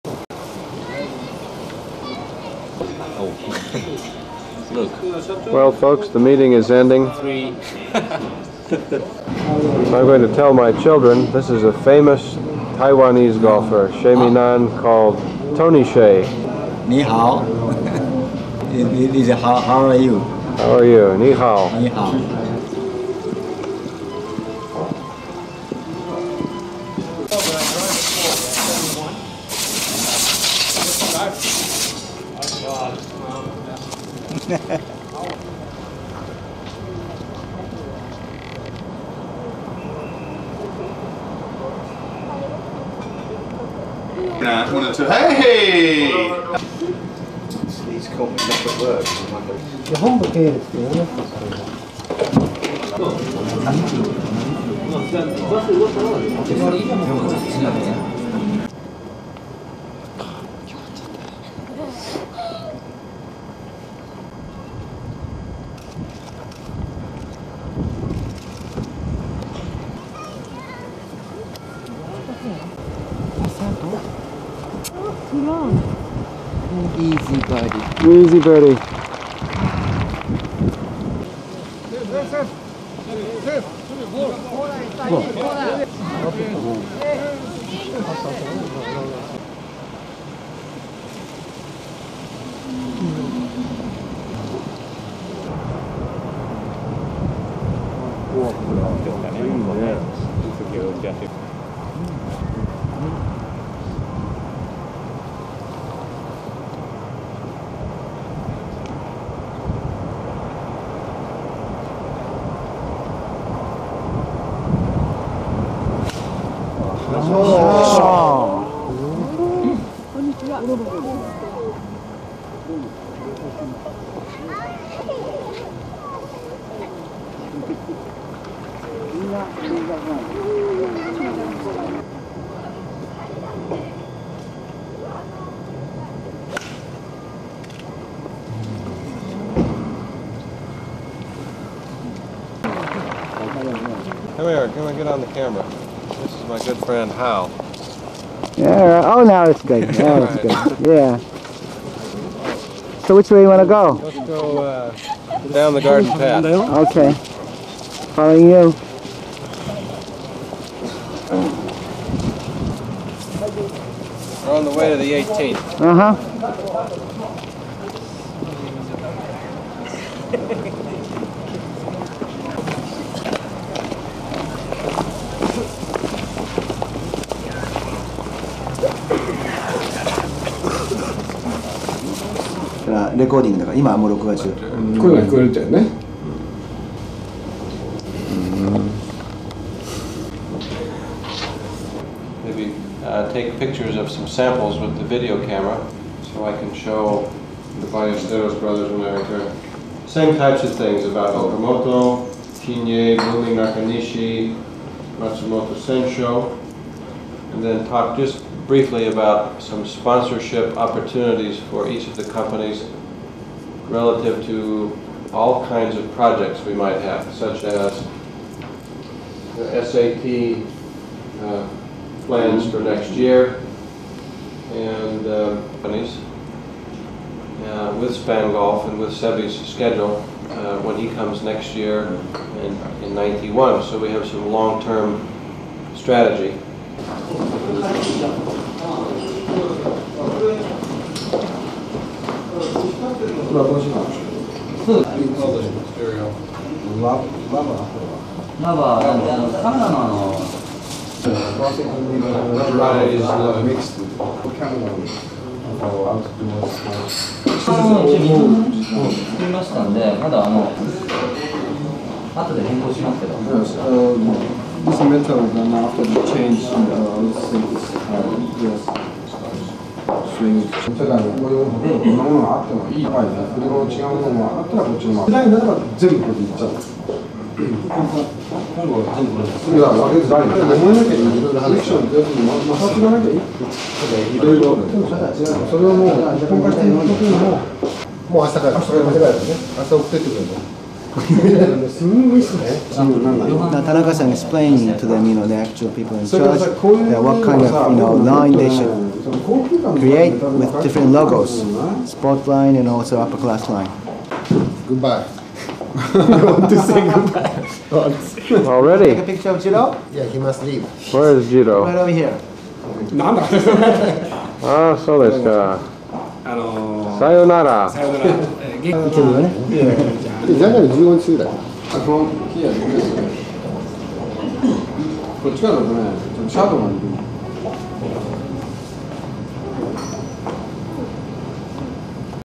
Dunlop phx short 1989
Joe Collet, Seve & Ajioka team at 1989 Dunlop Phoenix, Miyazaki, Japan